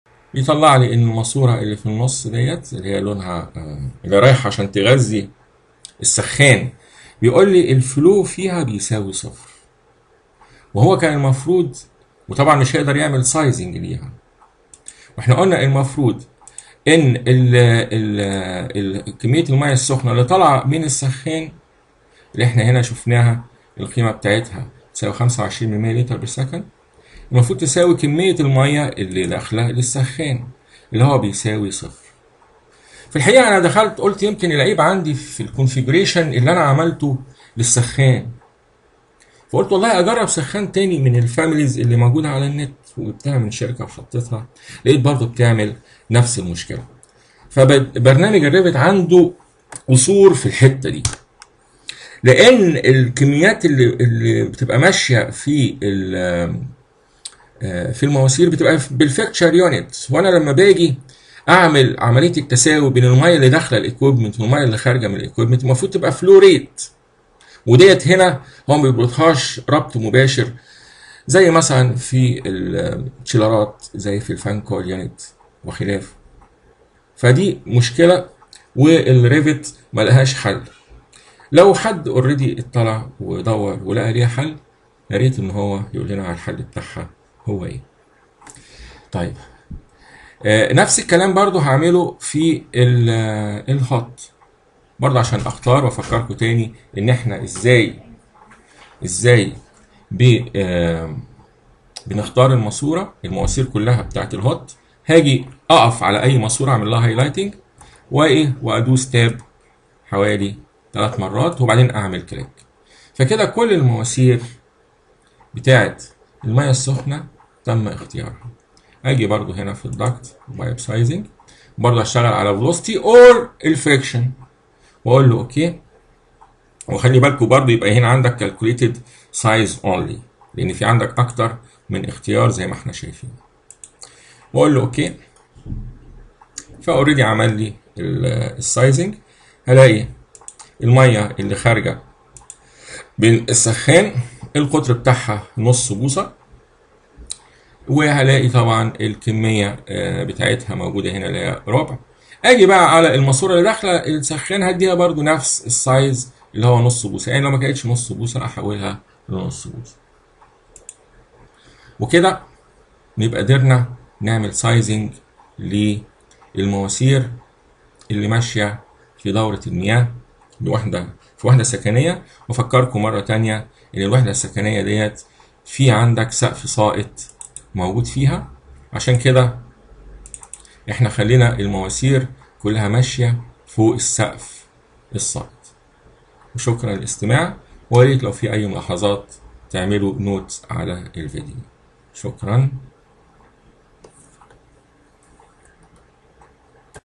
Domestic Cold/Hot Water Pipe Sizing Note 2
ملاحظات على تحديد أقطار مواسير التغذية بالمياه الباردة/الساخنة في المباني بإستخدام برنامج ريفت
الفيديو الثاني